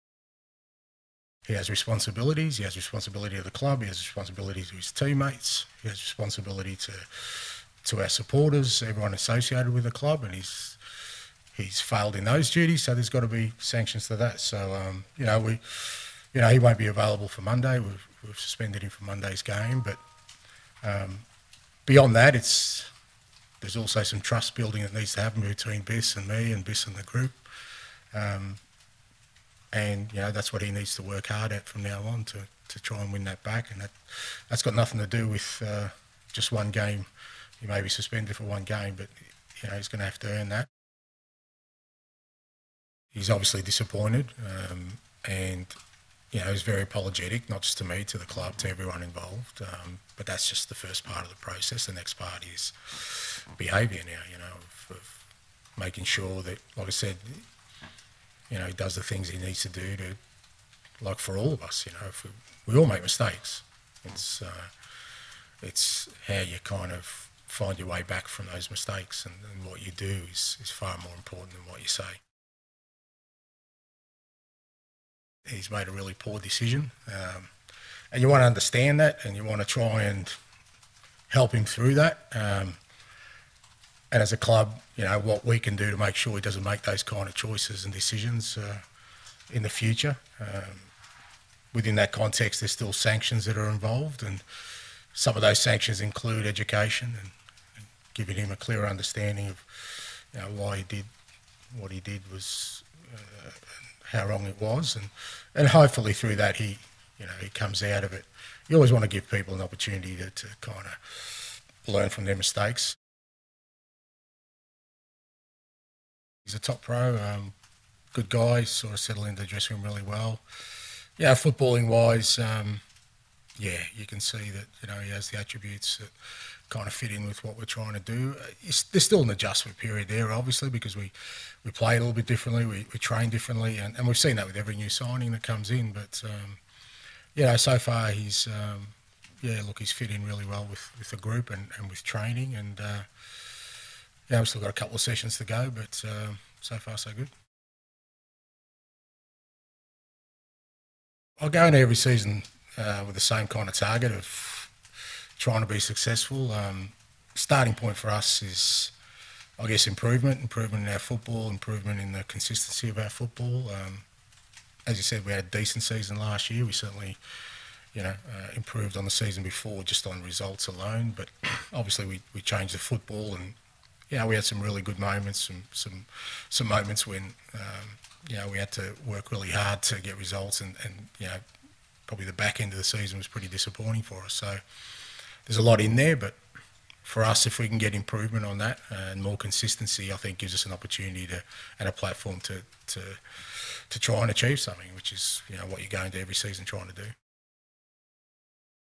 Postecoglou on Bissouma suspension: He has failed in those duties there has to be sanctions for that
Tottenham Hotspur head coach Ange Postecoglou revealed on Thursday that the north London club have suspended midfielder Yves Bissouma for their opening game of the new English Premier League season on Monday after footage emerged appearing to show the midfielder inhaling laughing gas.